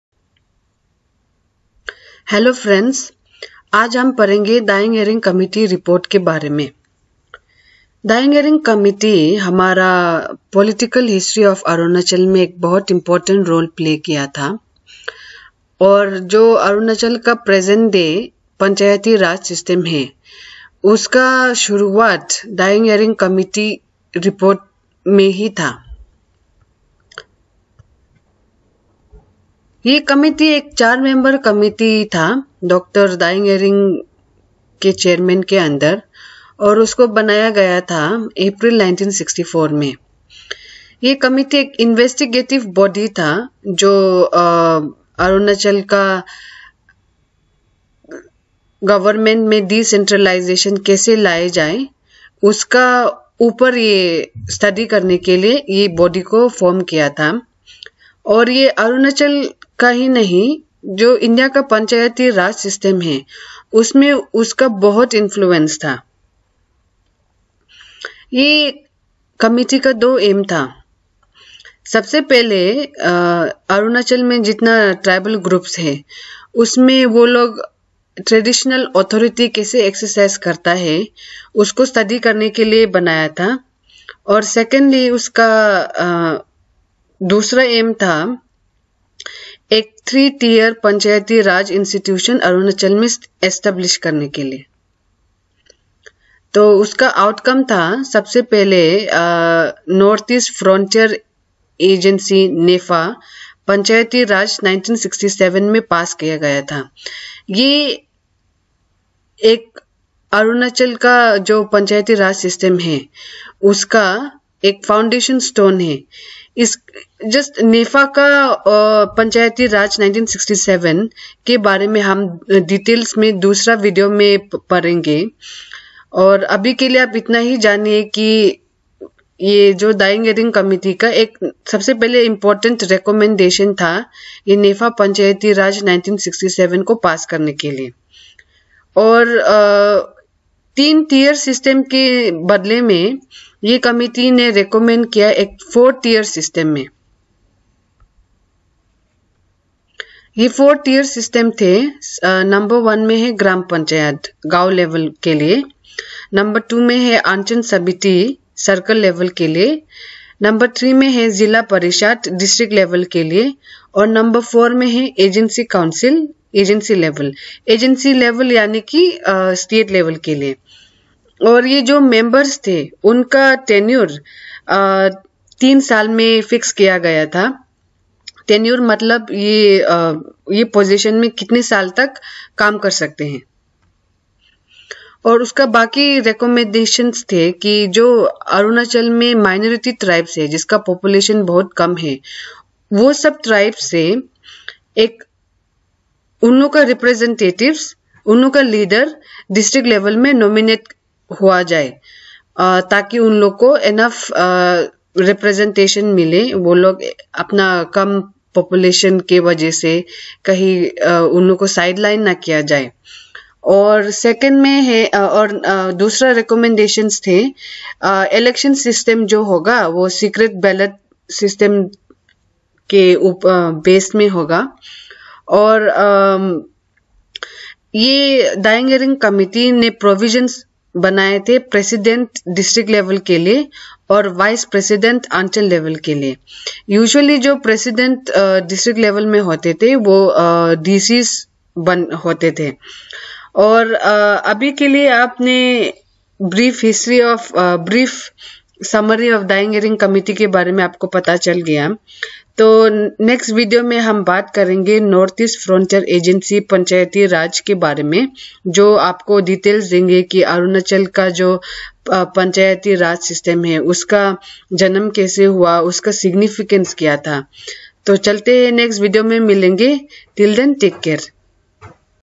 Daying Ering Committee 1964
This video is a summary of the Daying Ering Committee of 1964. Daying Ering Committee was a 4 member committee formed in April 1964 under the Chairmanship of Dr. Daying Ering. It was an investigative body looking into governmental decentralization. It influenced not only Arunachal Pradesh's Panchayati Raj System but India’s democratic decentralization.

Aim of the Daying Ering committee were:
1. Study the method through which authority was exercised by different indigenous tribal institution
2. Establish a 3 tier Panchayati Raj Institution
Outcome of Daying Ering Committee were:
1. NEFA Panchayati Raj Act 1967 was passed.
2. A four (4) tier Panchayati Raj System was introduced.
3. Minority tribes had nominated representatives at the district level
4.  Secret ballot system
5. Provisions of President at District level and Vice President at Anchal Level